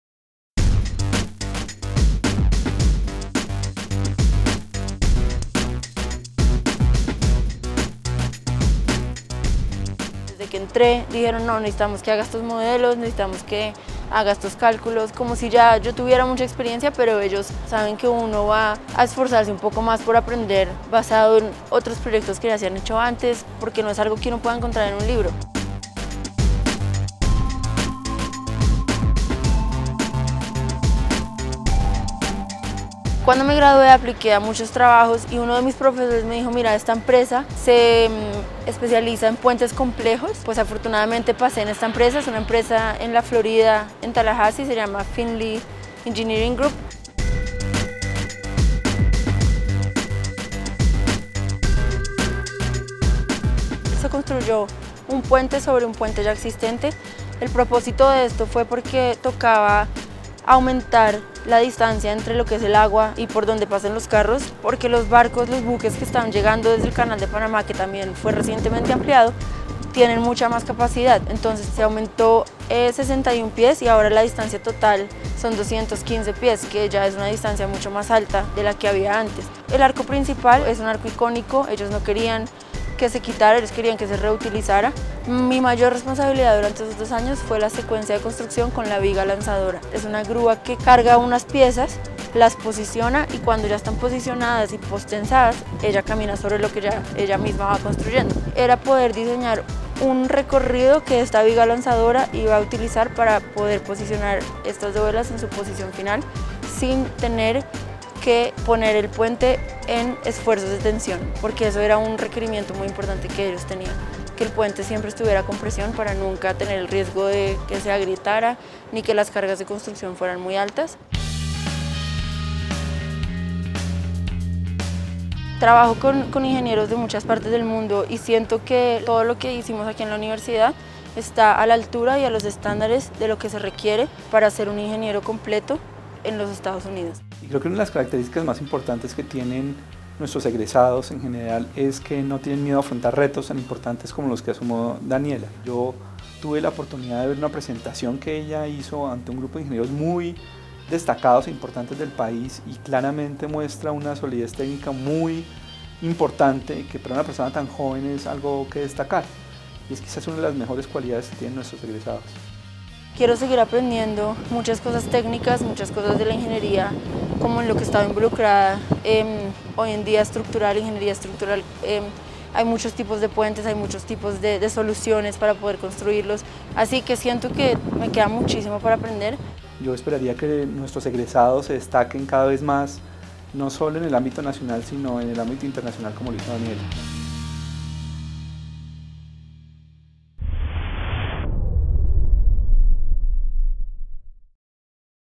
Colombian involved in the construction of the Bayonne Bridge in New York
Daniela Moreno, who studied civil engineering in Los Andes was part of the design and construction of the new Bayonne Bridge in Staten Island, New York. Due to the Panama Canal expansion, a new structure was required to raise the height of the bridge for larger ships that have a higher load capacity. As such, a new bridge was built above the old one, built in 1931, to avoid having to reroute the traffic that uses the bridge on a daily basis. The bridge now has been increased by a height of 61 feet.

It is hoped that in the future train tracks will be laid on the bridge.